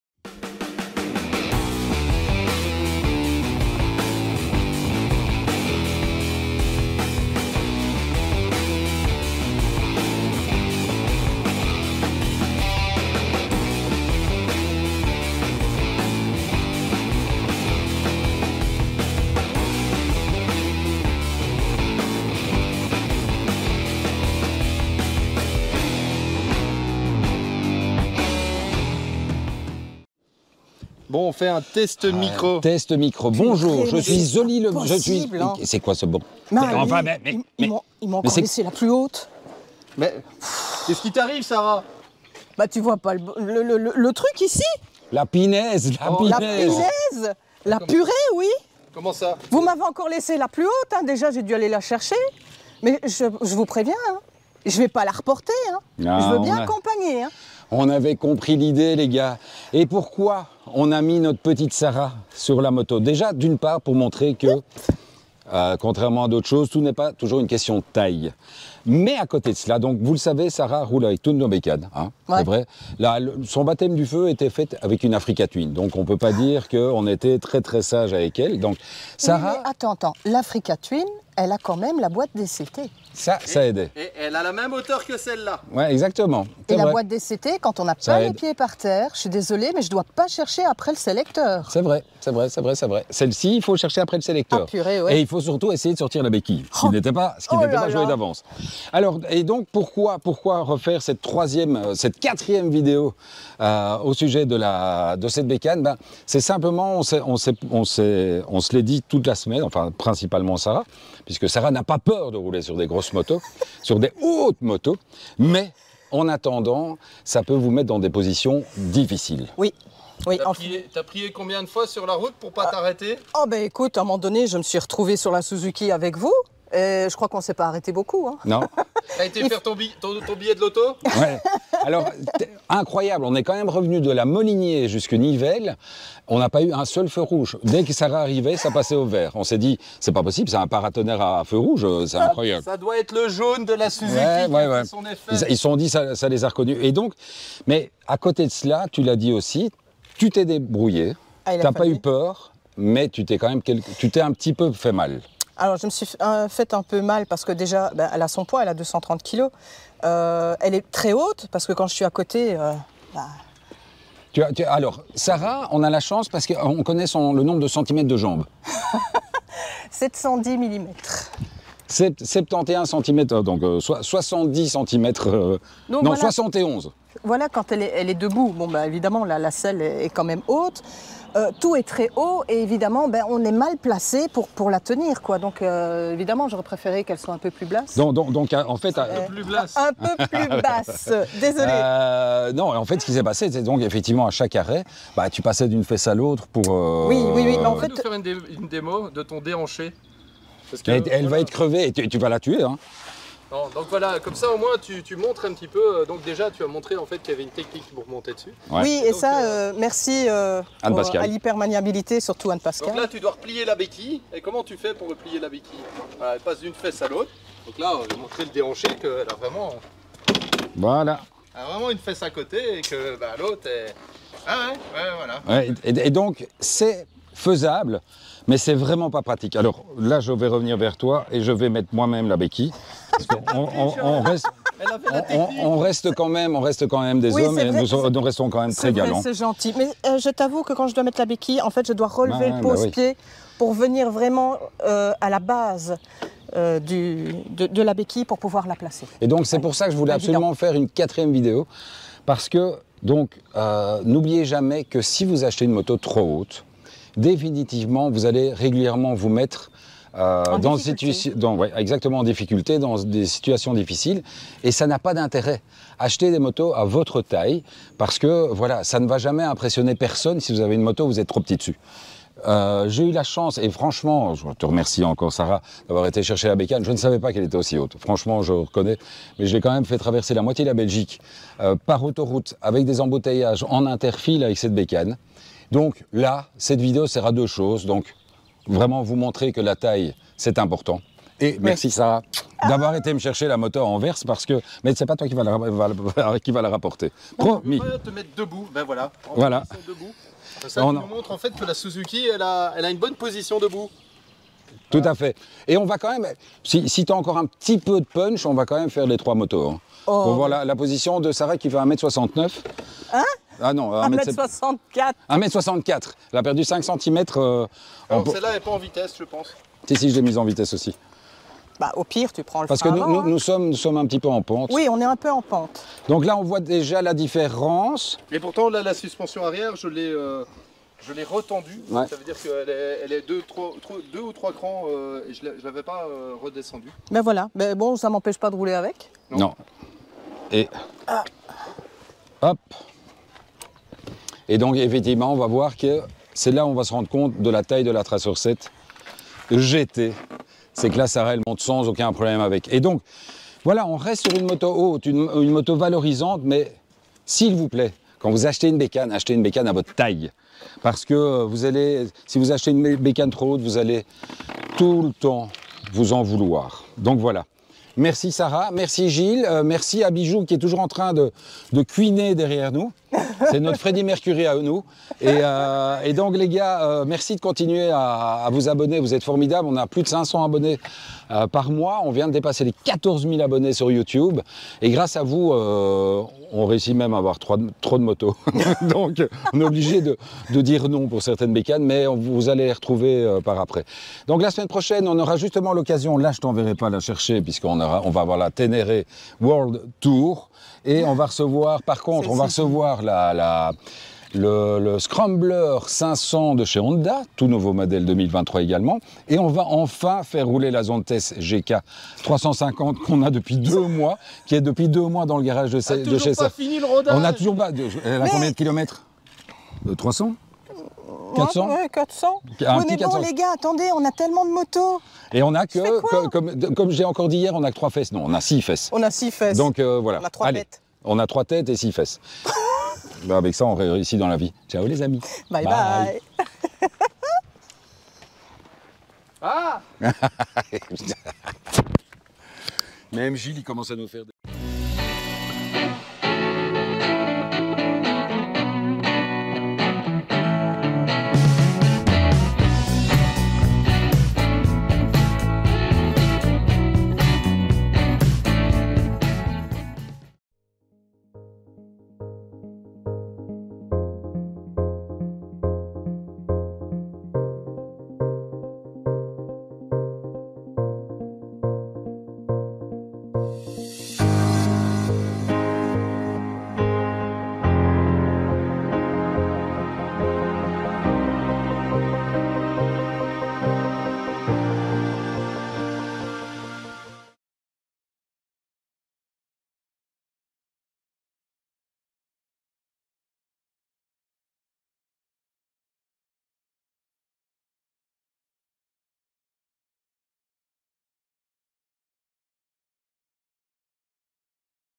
Yamaha Tracer 7 GT vs Honda transalp vs Suzuki Vstrom 800 DE épisode 3/3
Cette fois, on parle de l'expérience de Sarah, qui par les hasards du calendrier, a du aller chercher l'impressionnante VStrom 800 DE. Alors, la taille ça compte? Oui et non ;-) Episode 1 Episode 2